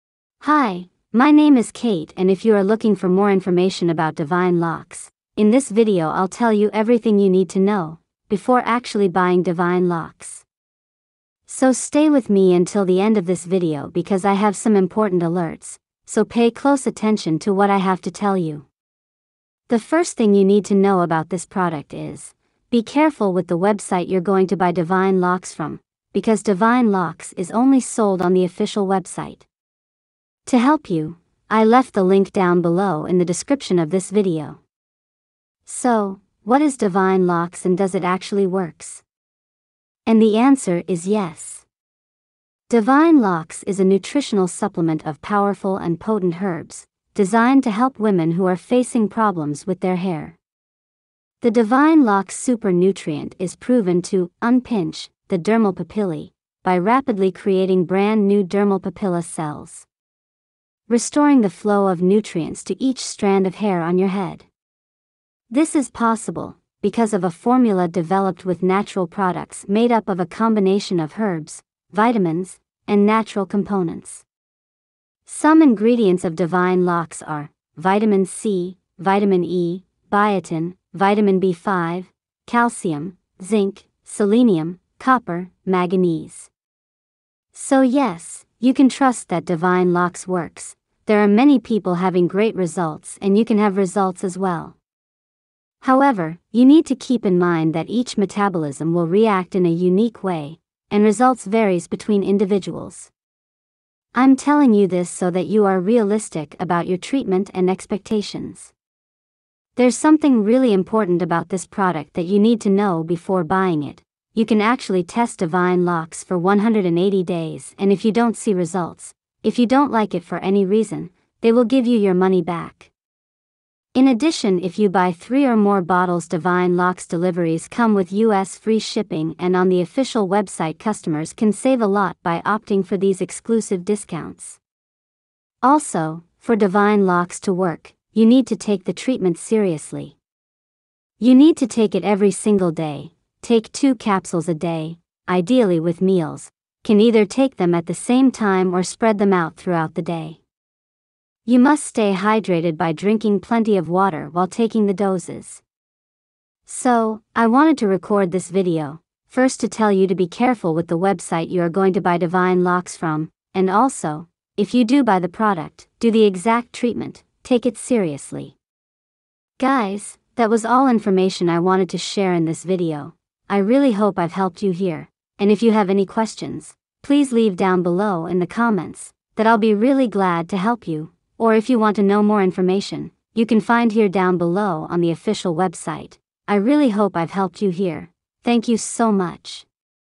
Does Divine Locks Actually Work ? Did you know that ?
💡What is Divine Locks ?

Divine Locks is a nutritional supplement of powerful and potent herbs designed to help women who are facing problems with their hair. The Divine Locks super nutrient is proven to “unpinch” the Dermal Papillae by rapidly creating brand new dermal papilla cells. Restoring the flow of nutrients to each strand of hair on your head. Divine Locks Complex is manufactured to the highest possible standard, In an FDA-registered, GMP-certified facility.

💡Does Divine Locks really work?

Yes, Divine Locks really works as it provides repair, volume growth and cellular regeneration. According to the official website, the time to see the benefits of Divine Locks on hair may vary, because take time for your Dermal Papillae cells to “unpinch” and proliferate. But not long! Studies show it takes about a week for your cells to see a difference. As your Dermal Papillae cells grow, more nutrients and oxygen will be able to reach your hair follicles. Other benefits that can be noticed after use are:

Reverses signs of aging: Divine Locks has the ability to reverse many of the visible signs of hair aging that most women are faced with after they cross 30.

Prevents receding hairline: An underrated aspect of this product is that it seeks to help mitigate many of the issues that arise due to the natural aging process.

Natural composition: All of the ingredients present in Divine Locks are natural in their composition and be totally biocompatible with the human system.

✅Every customer who buys a bottle of Divine Locks receives a 180-day money-back guarantee.

💡Ingredients: divine locks is made from 28 natural ingredients, any of these ingredients are:Vitamin C, Vitamin E, Biotin, Vitamin B5, Calcium, Zinc, Selenium, Copper, Maganese

💡 How often should I take it?

You take two capsules a day, ideally with meals. You can either take them at the same time or spread them out throughout the day.

💡Benefits and side effects Divine Locks As it is a 100% natural supplement, it has no side effects. Some of its benefits are:

✅ Improved dermal papillae production

✅ It improves your hair shine, strength and texture

✅ Improve hair density

As I said in this Divine Locks review, everyone's metabolism works differently and you need to know that it is not a miracle supplement. However, take the treatment seriously following the instructions for use, it is possible to notice a great improvement and benefits for your hair. I hope I can clear up all your doubts about the Divine Locks review, do the treatment correctly and don't hesitate to ask!